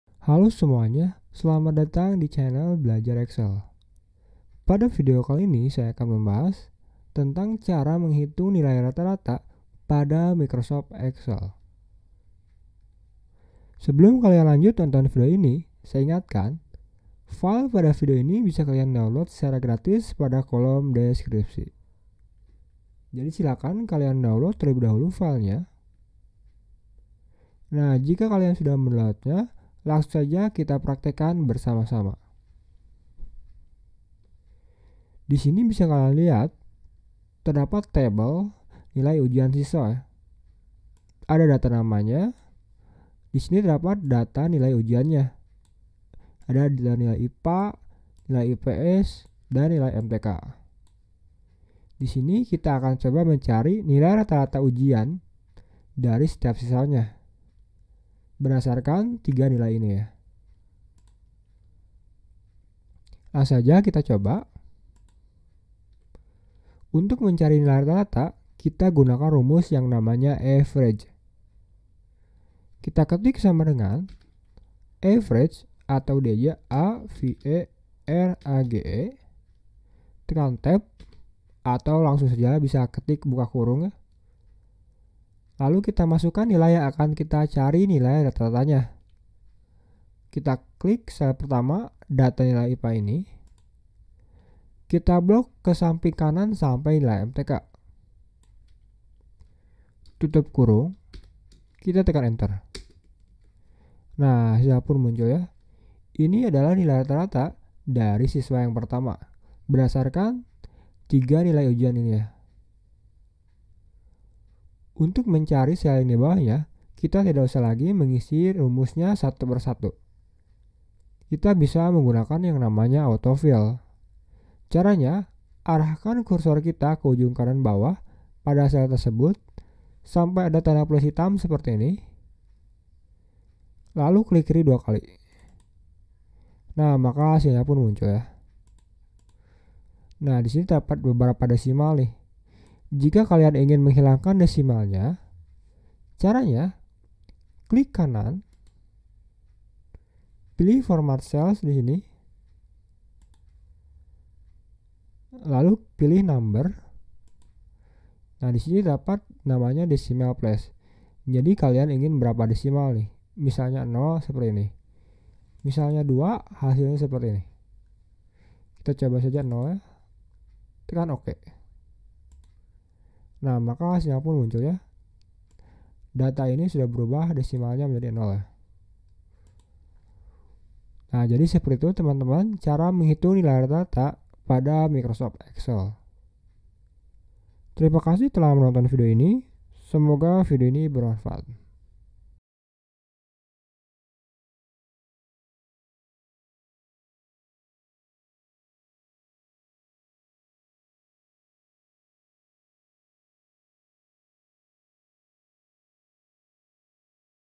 Cara Menghitung Nilai Rata - Rata di Excel
Video ini bermanfaat bagi semua orang yang tertarik untuk mempelajari Microsoft Excel.

Dalam video ini, Teman - Teman akan mempelajari cara menghitung nilai rata- rata agar mempermudah pekerjaan tersebut menggunakan Microsoft Excel. ️ 🔥🔥🔥🔥🔥
✔️ Menghitung rata - rata
✔️ Rumus AVERAGE